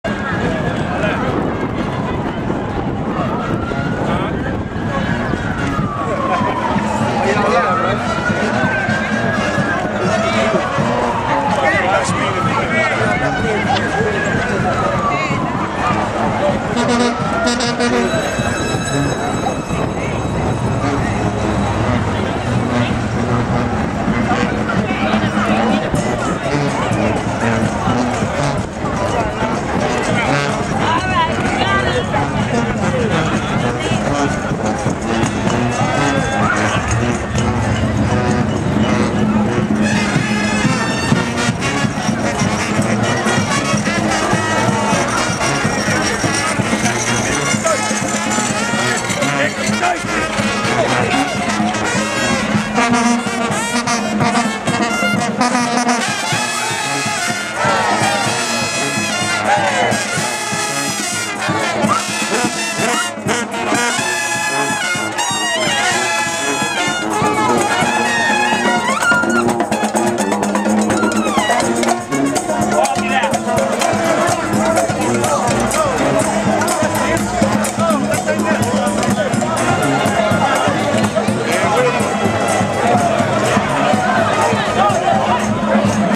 Revolution S&P '10 2nd Line (077); Super Sunday
Revolution Social Aid & Pleasure Club's second lining from the 7th Ward to Central City, coming up Oretha Castle Haley Blvd.; documented by M. Darryl Woods of The Contraflow Project